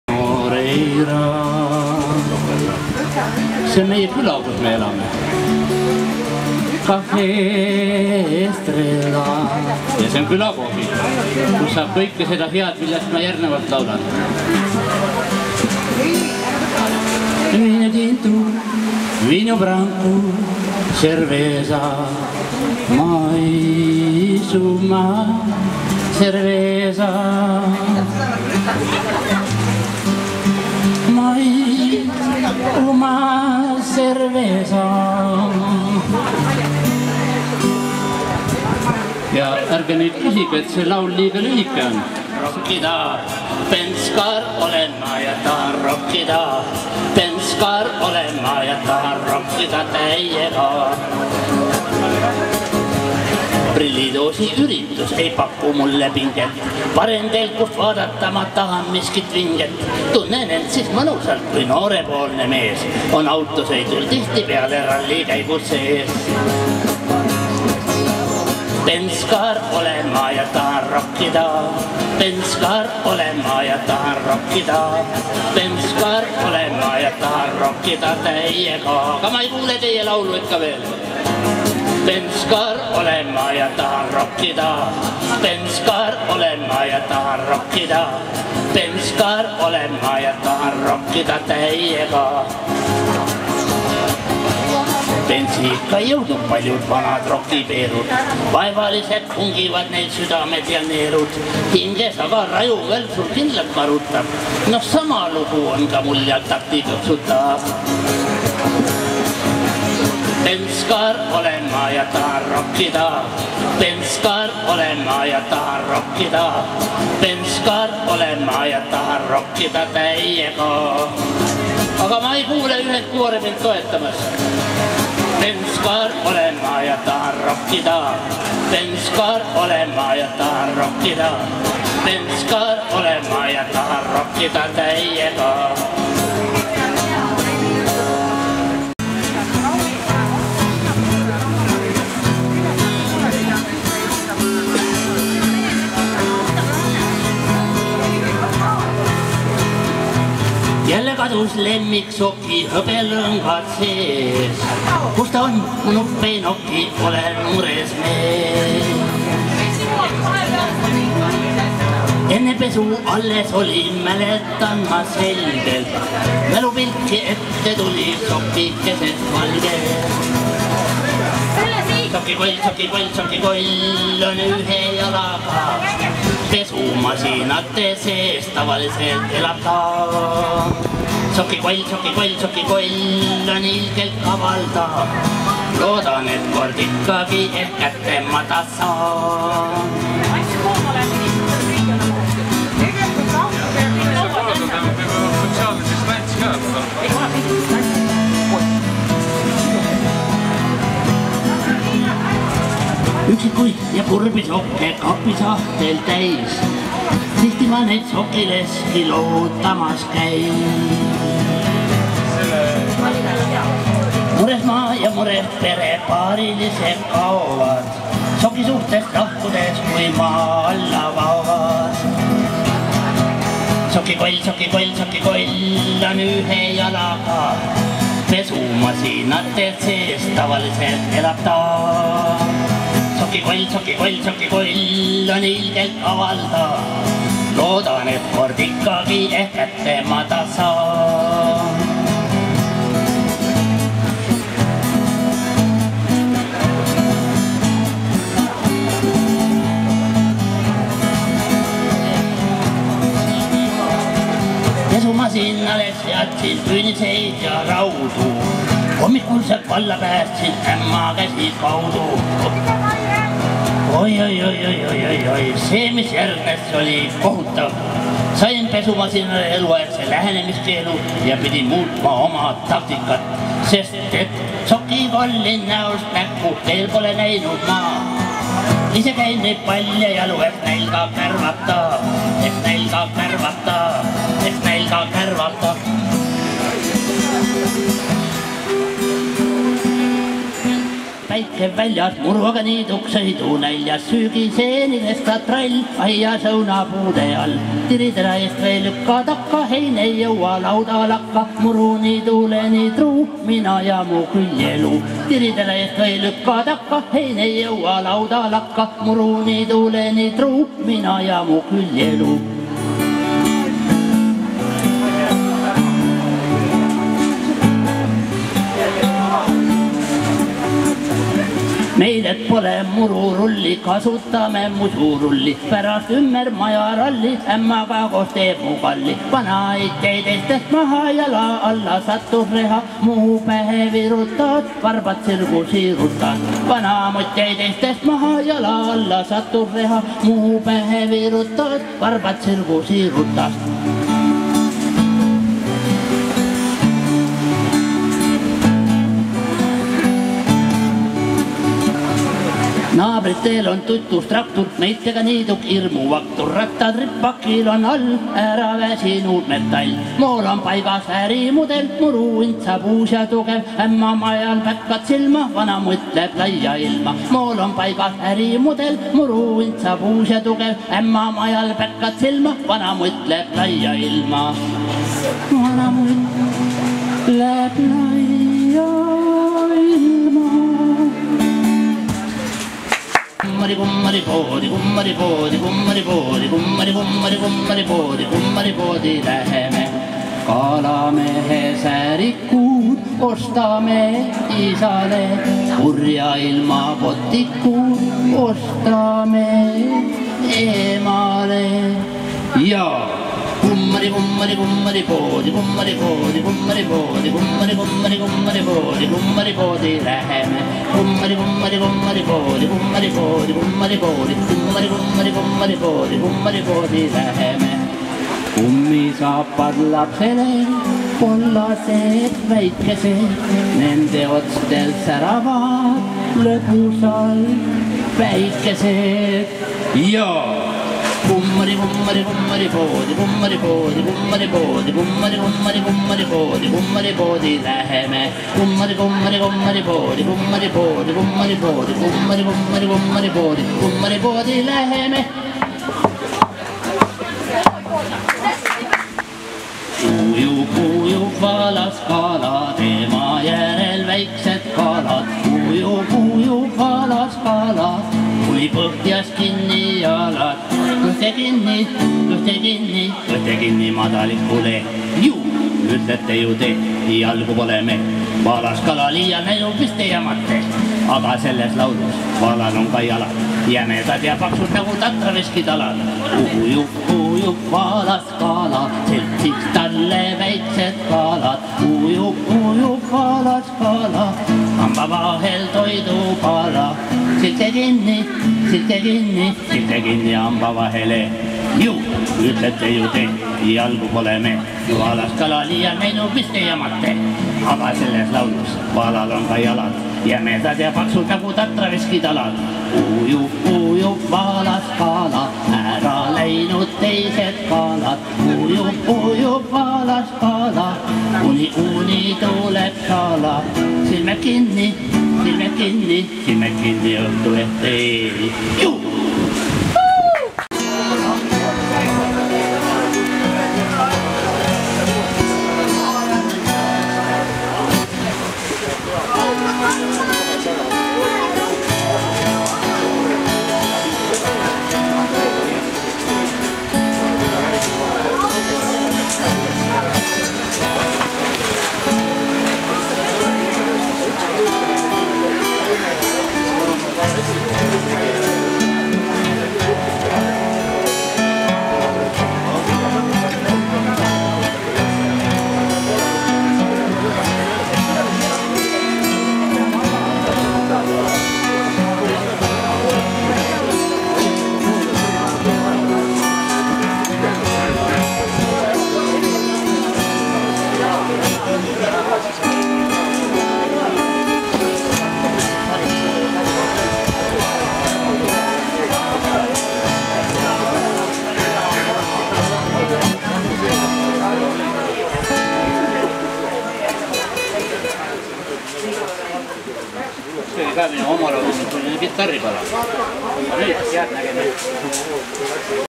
Aimar Roomets kitarri ja omaloominguga
Sadama täika Virtsus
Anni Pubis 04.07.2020